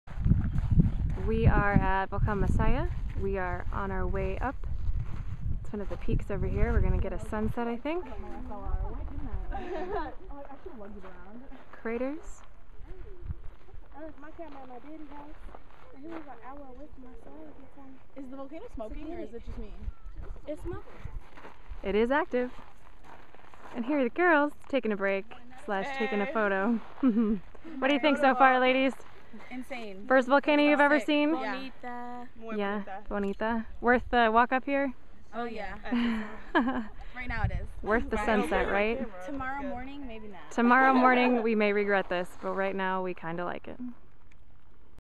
Climbing a Volcano
After driving to the first crater, the group decided to climb up to one of the peaks for a better view of the sunset and the valley.